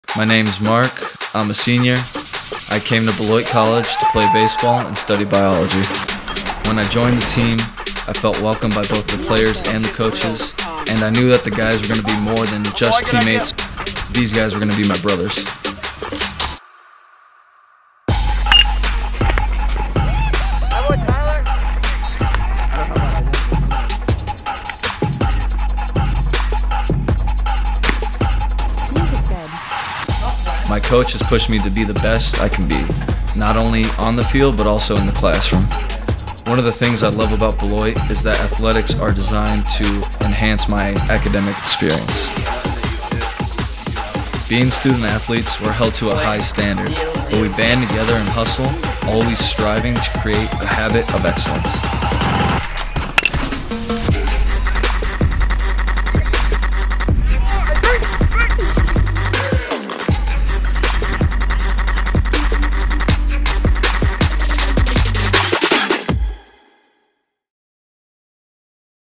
Beloit College Baseball
What’s it like to be a student-athlete at Beloit? Mark Christine’18 let cameras follow him around for a day, as he strives for excellence both inside the classroom and outside on the baseball diamond.